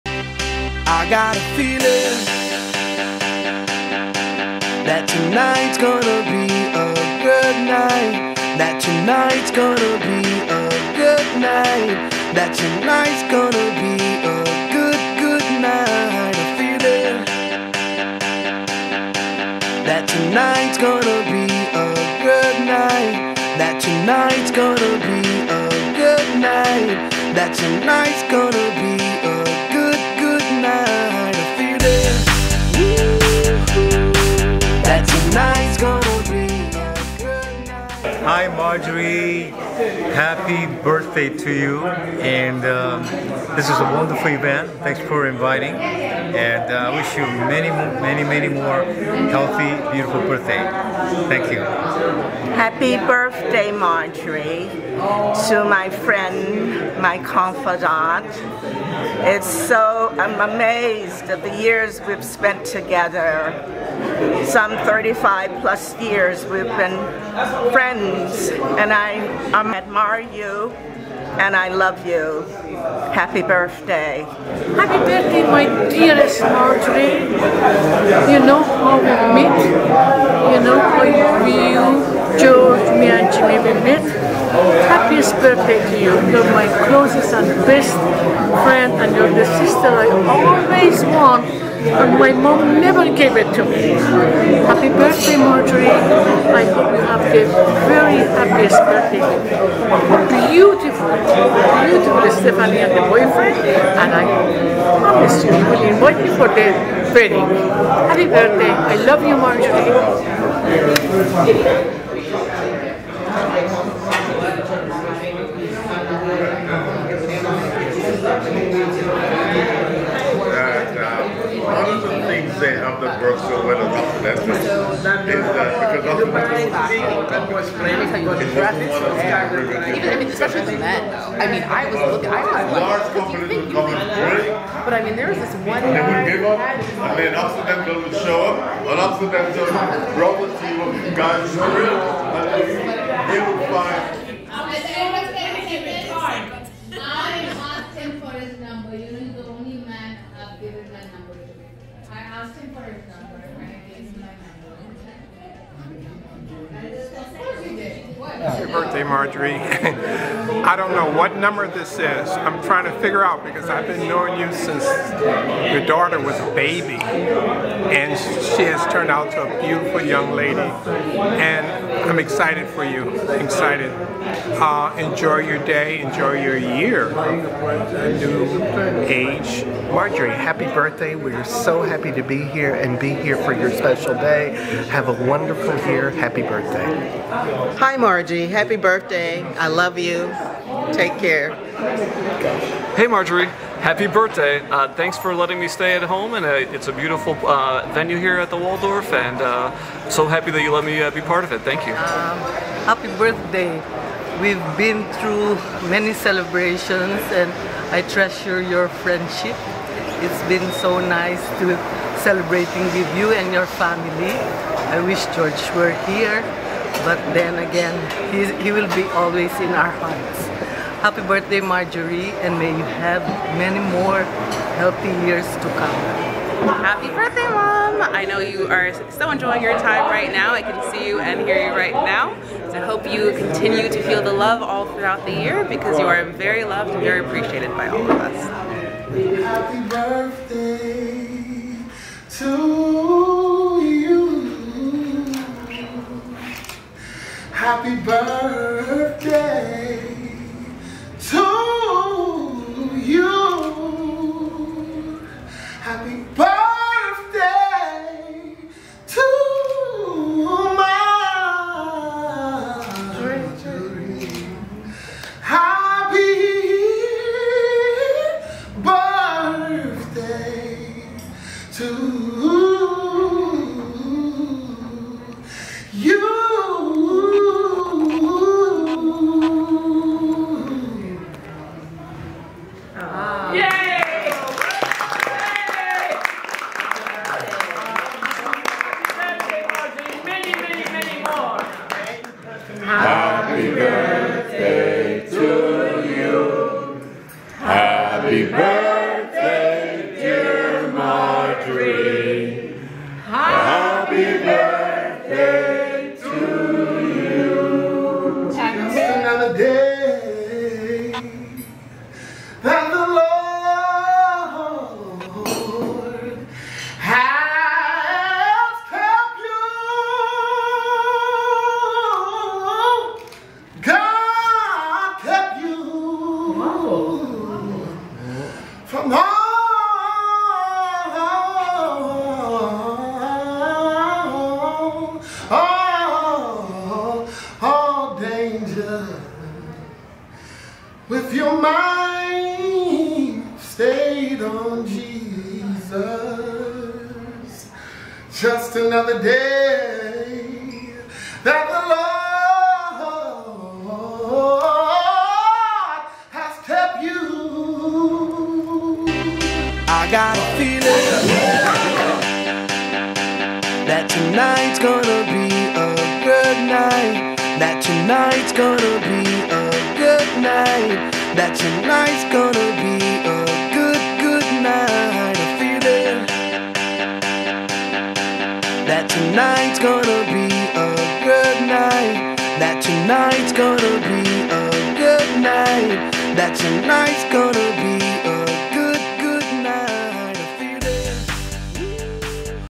Ambassador Marjorie Grant Fuller Birthday at Waldorf Astoria
Ambassador Marjorie Grant Fuller Birthday Celebration at the Waldorf Astoria in Beverly Hills.
Photographer/Videographer Stevesimage